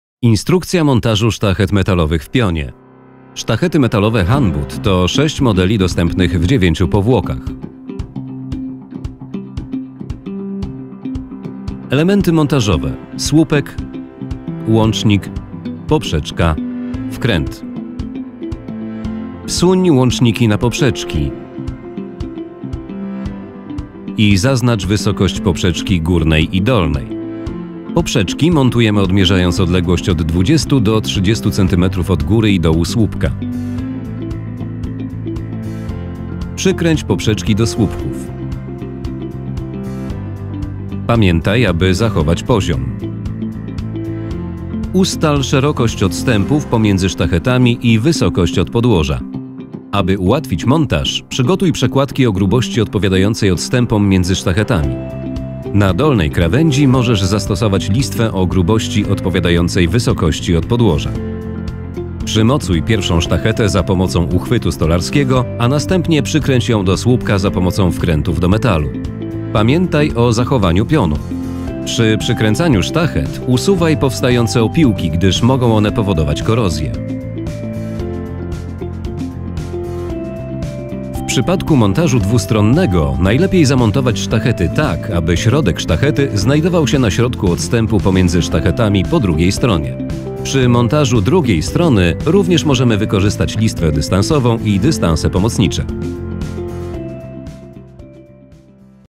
Instrukcja montażu sztachet metalowych Hanbud w pionie - jednostronnie i dwustronnie
Instrukcja montażu sztachet metalowych w pionie pokazuje, jak w prosty sposób, przy wykorzystaniu podstawowych narzędzi stworzyć gotowe przęsło.